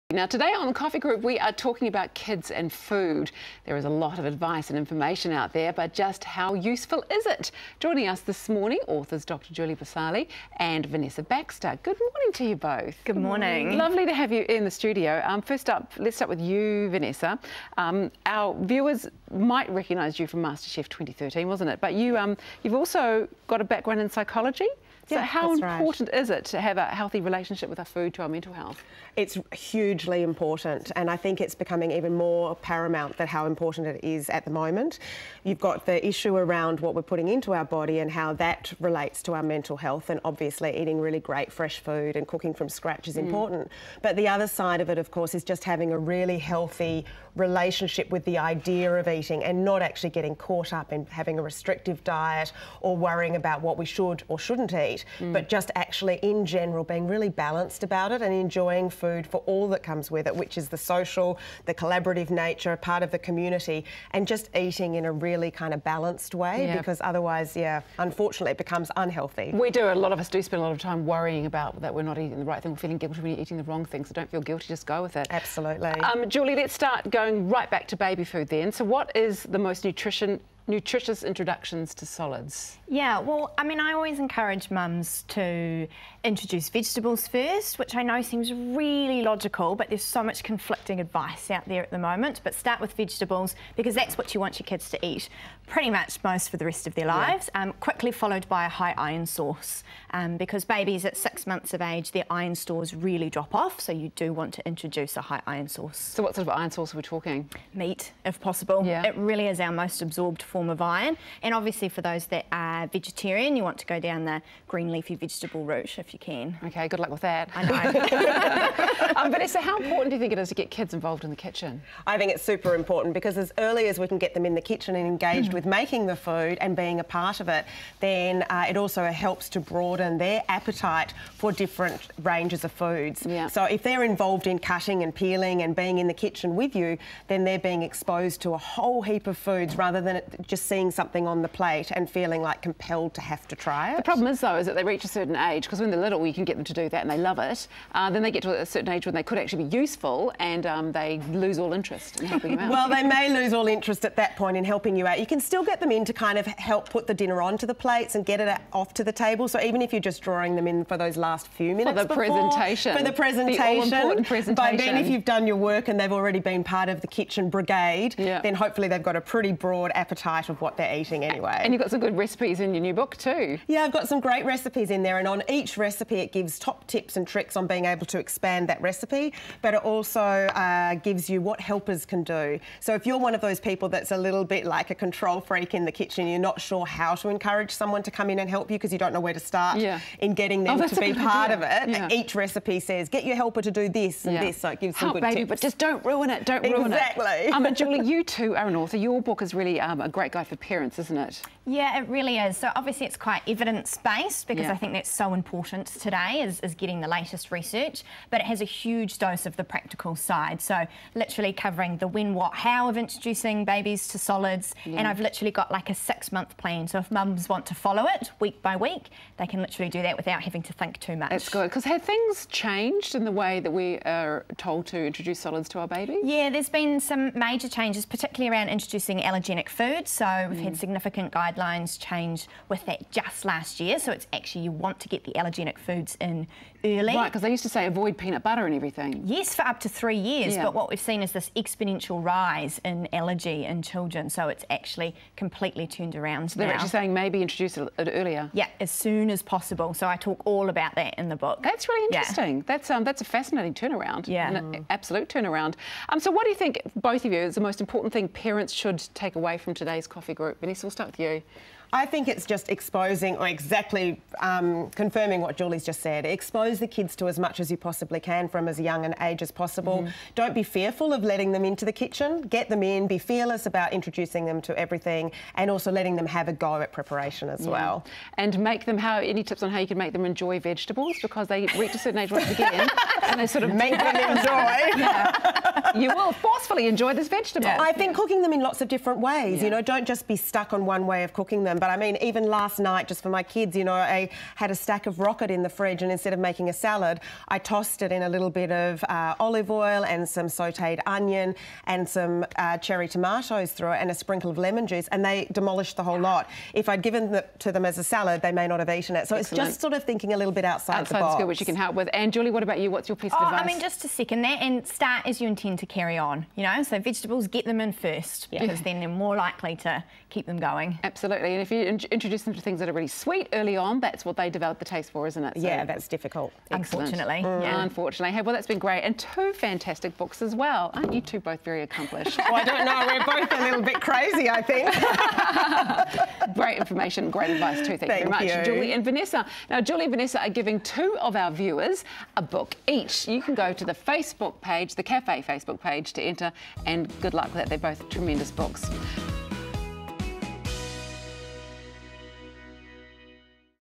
Get kids excited about good food with authors Dr Julie Bhosale and Vanessa Baxter
How to get kids excited about good food from an early age with authors Dr Julie Bhosale and Vanessa Baxter.